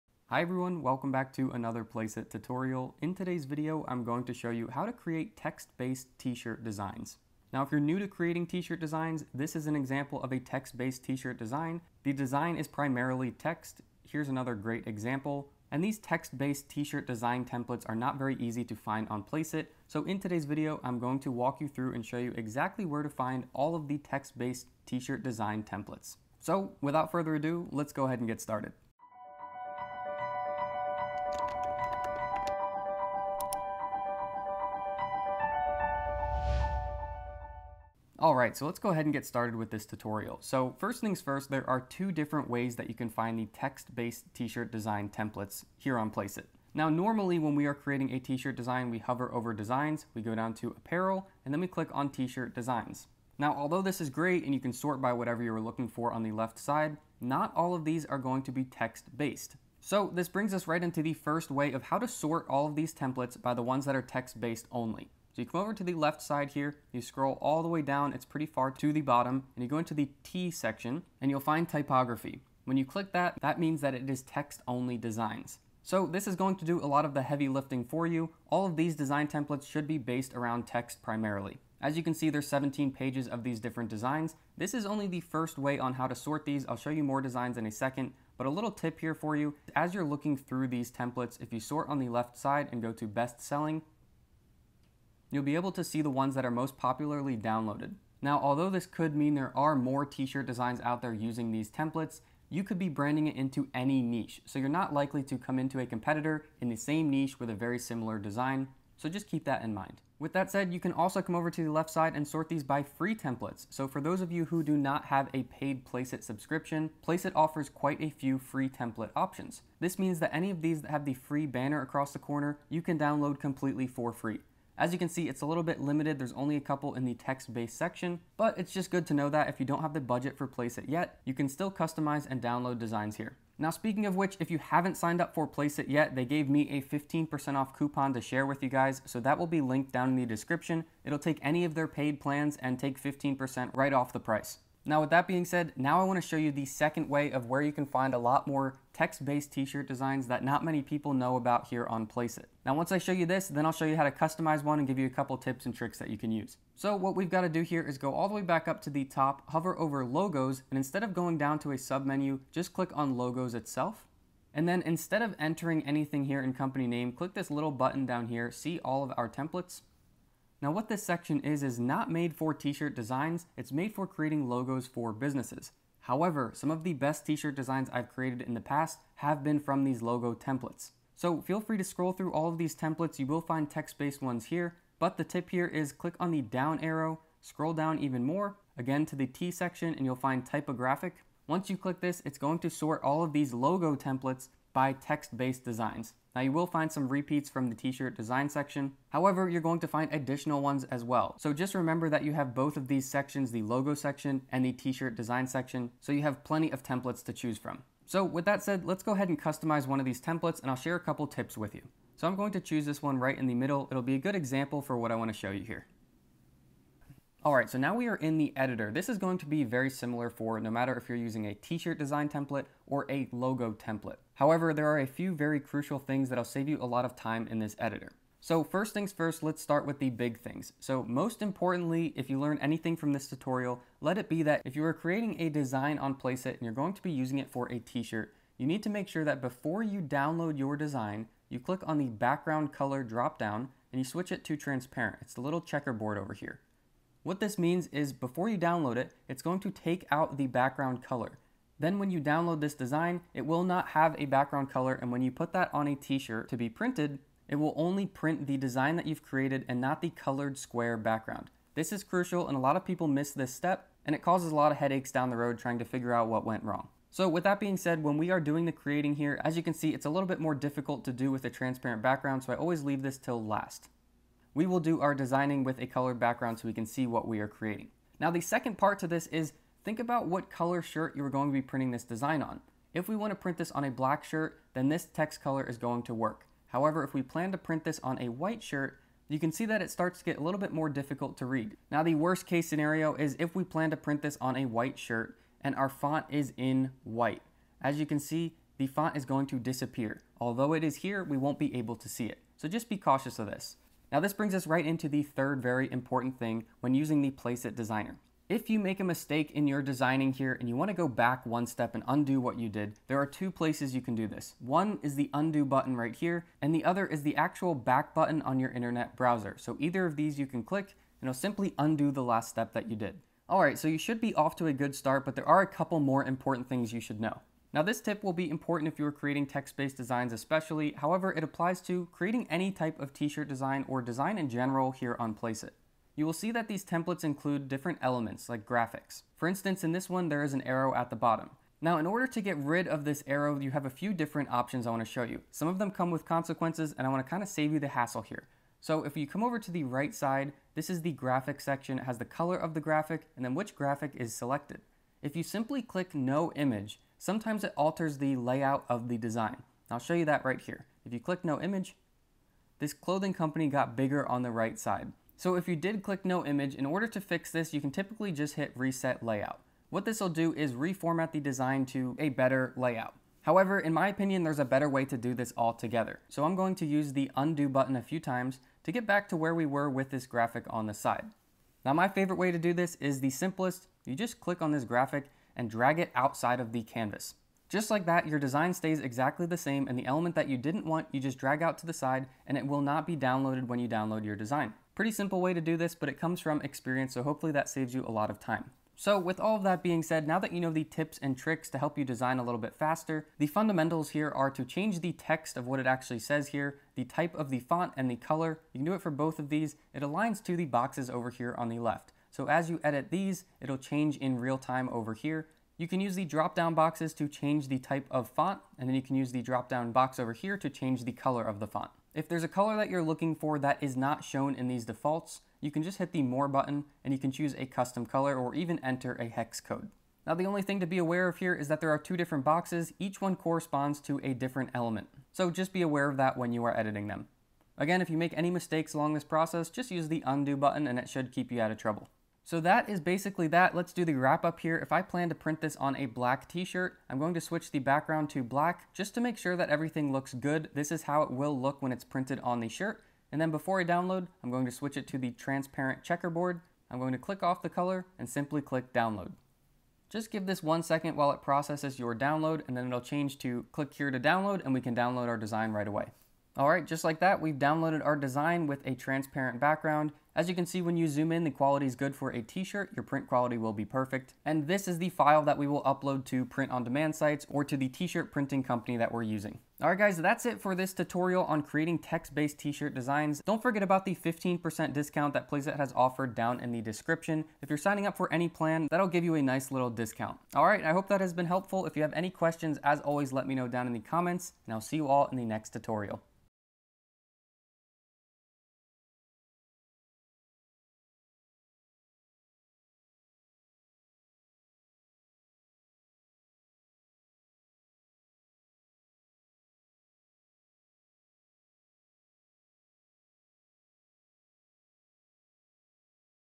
How To Make Text T-Shirt Designs | Placeit Tutorial
In this Placeit tutorial, learn how to use Placeit to make text based t-shirt designs that sell. Placeit is known for it's t-shirt design templates, however finding "text only t-shirt designs" is not so easy. This tutorial will show you step-by-step how to make text only t-shirt designs on Placeit.